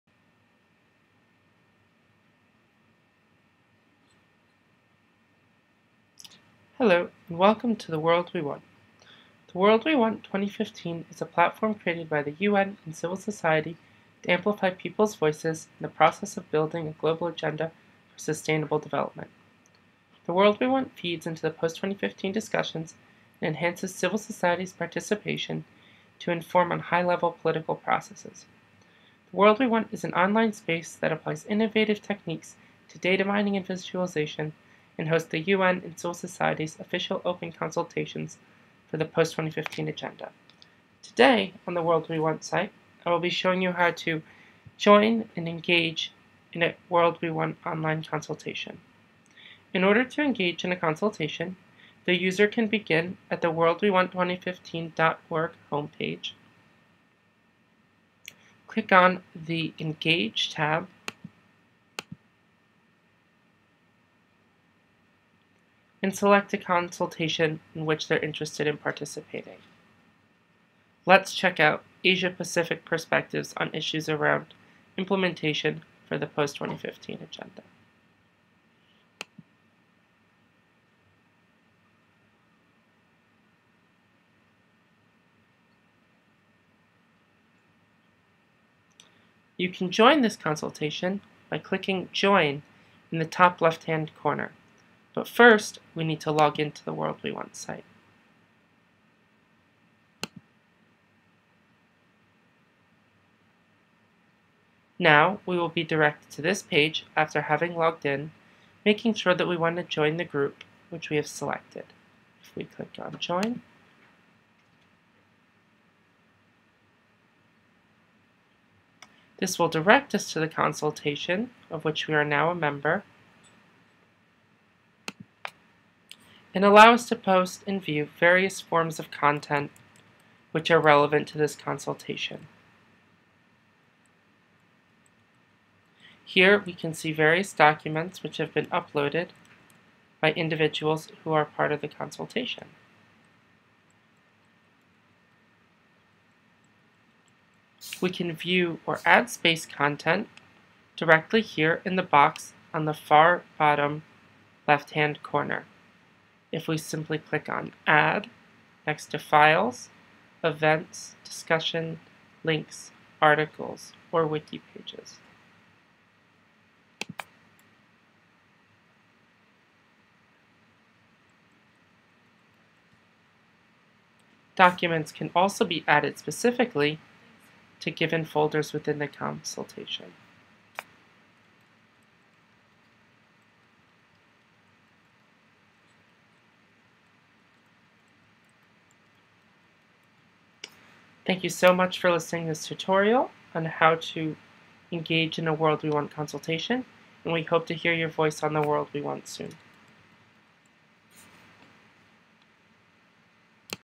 Tutorial for how to engage and join, an online consultation on the World We Want 2015 platform
This video describes the manner in which to participate in the variety of online conversations hosted on the World We Want 2015